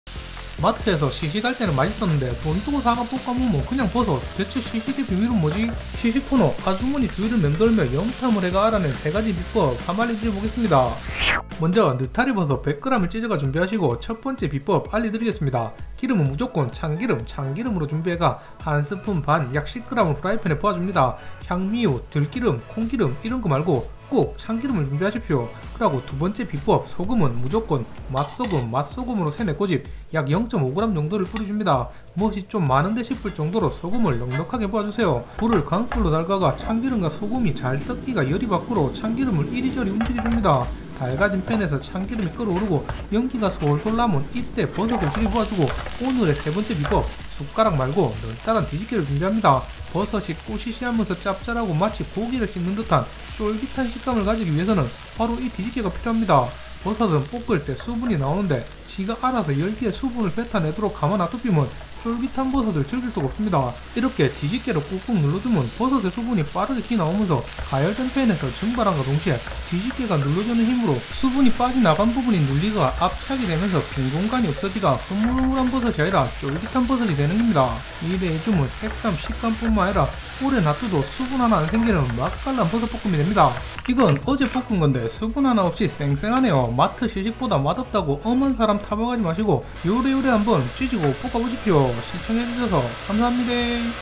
시식코너에선 맛있었는데 내가 볶으면 그냥 버섯인 이유~ 세가지만 알려드립니다~ 느타리버섯, 버섯 레시피
시식코너는 맛있고 내가 볶으면 그냥 버섯인 이유~

집밥을 맛있게 찌지고 뽁는 두율이네의 맛있는 이야기~

1호기도 율~ 2호기도 율~ 그래서 두율이네!!
두율이네 맛있는 집밥 이야기로 함 달려보입시다!!